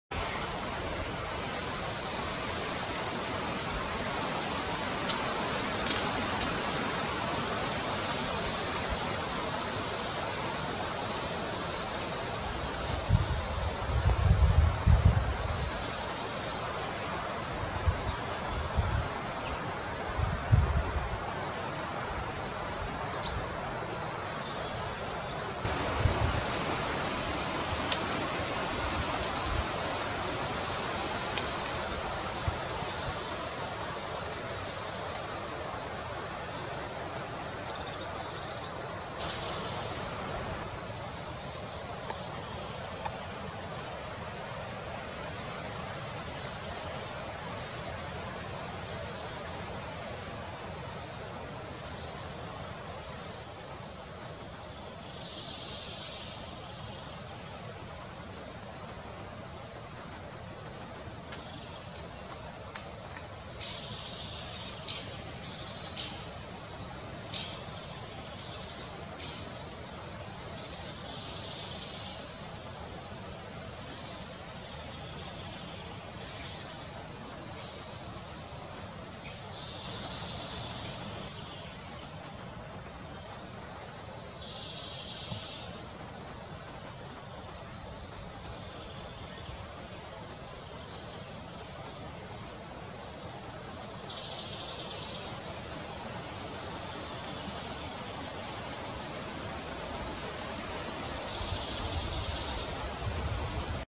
Grass snake (Natrix natrix) - Natrice dal collare
Grass snakes (Natrix natrix) recorded in Kampinos National Park, Poland (June, 2020).

Natrici dal collare (Natrix natrix): video registrati nel parco Nazionale di Kampinos, in Polonia (Giugno, 2020).